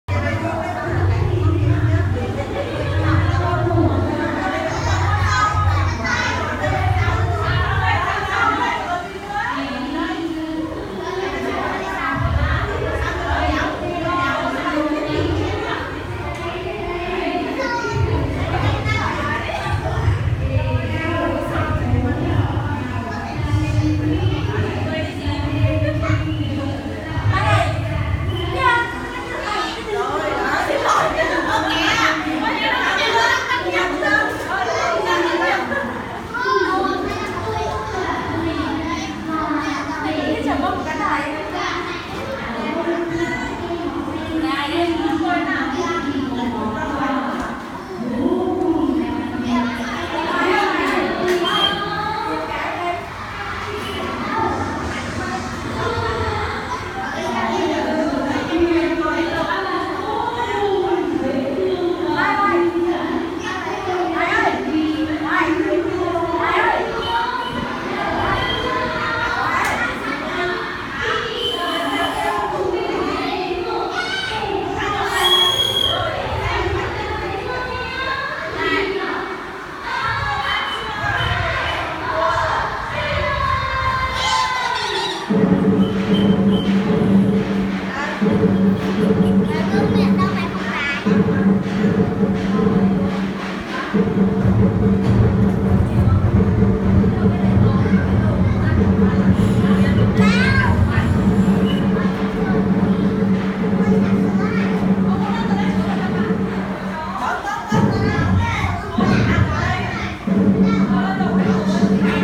Trung thu của các bạn 2A1 Lê Văn Tám
Đón tết trung thu của các bạn học sinh lớp 2A1 Lê Văn Tám thật vui